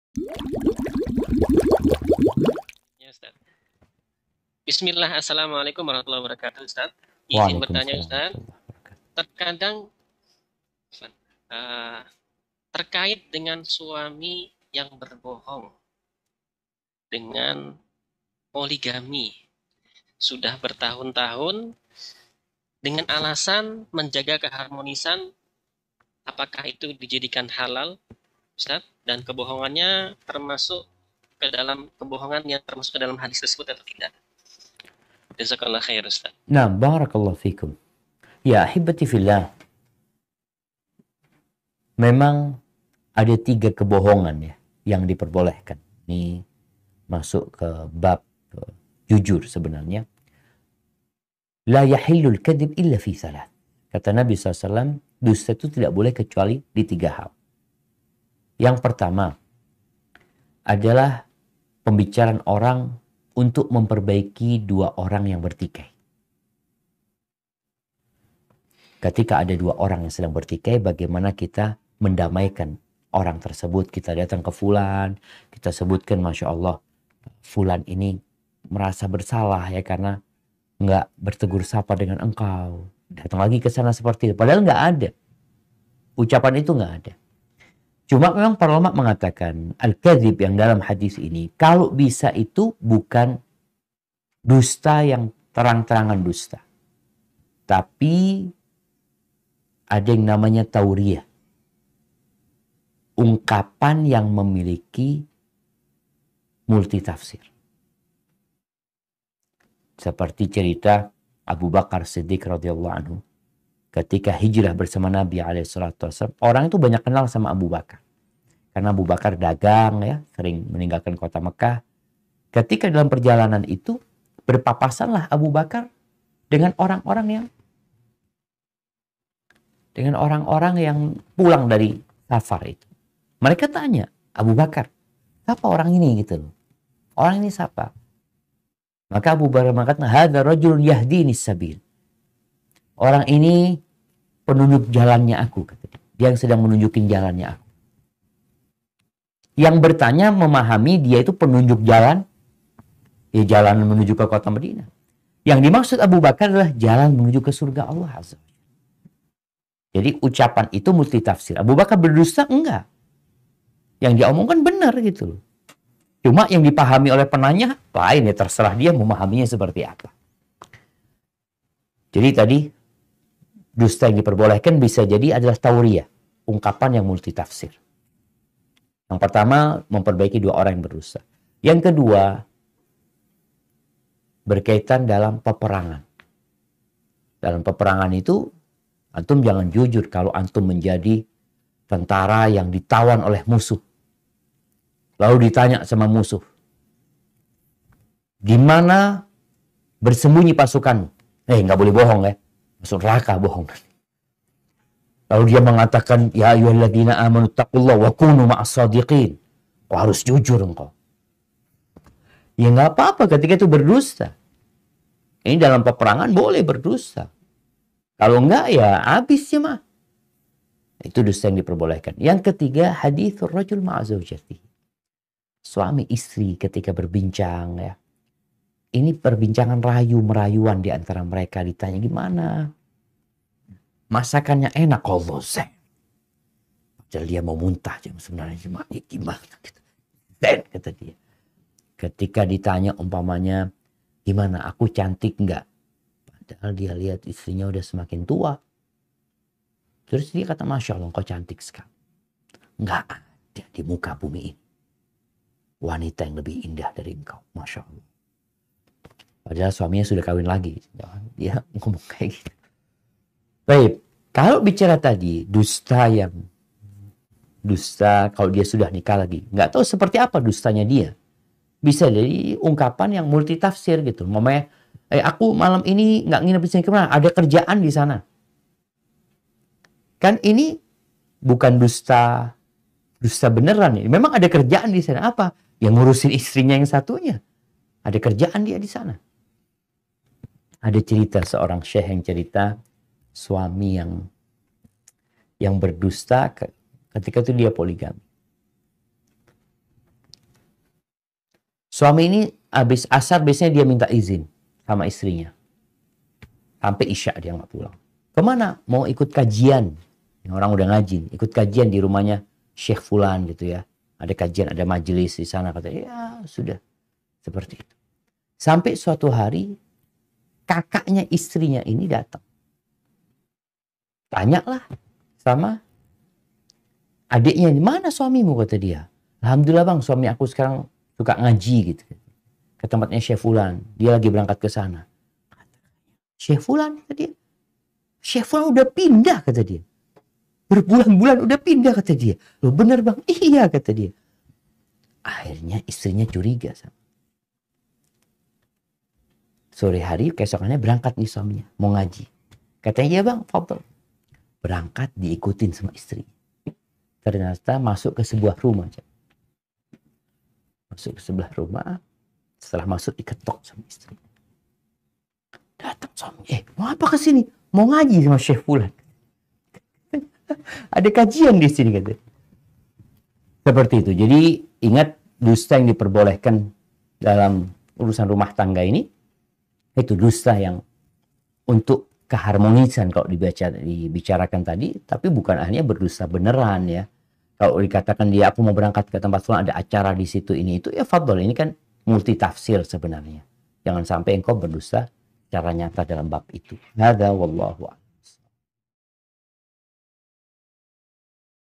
3 HAL yang Membuat Muslim Boleh Berbohong (Tauriyah) - Dr. Syafiq Riza Basalamah, Lc, MA
3 HAL yang Membuat Muslim Boleh Berbohong (Tauriyah)

Dijawab oleh Ustadz Dr. Syafiq Riza Basalamah, Lc, MA melalui channel resmi beliau @SyafiqRizaBasalamahOfficial